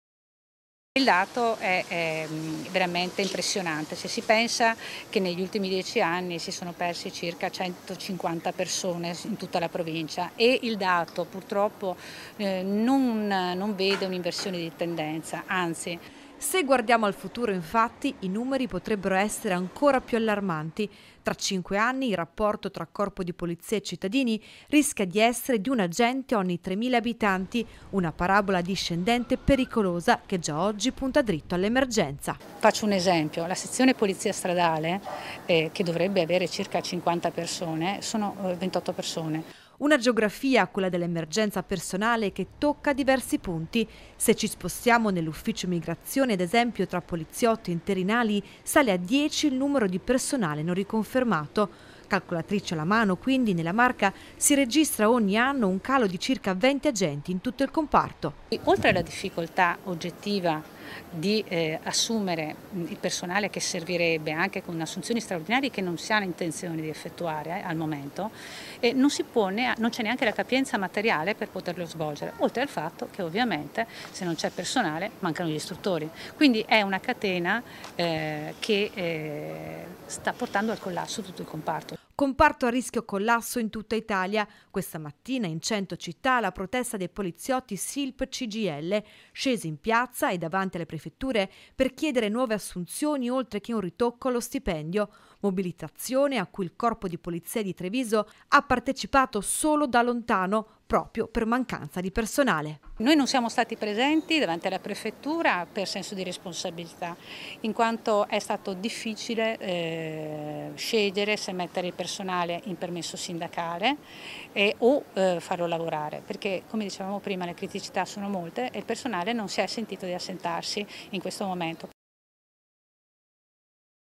POLIZIA, ORGANICI AL COLLASSO: NELLA MARCA 10 AGENTI IN MENO OGNI ANNO | 12/07/2023
A3 NEWS Treviso 12/07/2023 - TREVISO - Nella Marca comparto di Polizia a rischio collasso. Senza cambi di direzione, la mancanza di personale porterebbe tra cinque anni ad un rapporto tra corpo di polizia e cittadinanza, pari ad 1 agente ogni 3 mila abitanti. E questa mattina la protesta in tutta Italia degli agenti Silp Cgil.   || Tra 5 anni il rapporto tra corpo di polizia e cittadini, rischia di essere di 1 agente ogni 3 mila abitanti, una parabola discendente pericolosa che già oggi punta dritto all'emergenza. Una geografia quella dell'emergenza di personale che tocca diversi punti. Se ci spostiamo nell'ufficio immigrazione ad esempio, tra poliziotti e interinali, sale a 10 il numero di personale non riconfermato. Calcolatrice alla mano quindi, nella Marca, si registra ogni anno un calo di circa 20 agenti in tutto il comparto. Comparto a rischio collasso in tutta Italia. Questa mattina, in cento città, la protesta dei poliziotti SILP-CGIL, scesi in piazza e davanti alle Prefetture per chiedere nuove assunzioni oltre che un ritocco allo stipendio.  Mobilitazione a cui il corpo di Polizia di Treviso, ha partecipato solo "da lontano" proprio per mancanza di personale. - Intervistati GIOVANNA GAGLIARDI (Segretaria Provinciale Silp-Cgil di Treviso) (Servizio di Sharon Di Carlo) - Segui Antenna Tre anche sul digitale terrestre! Visita il sito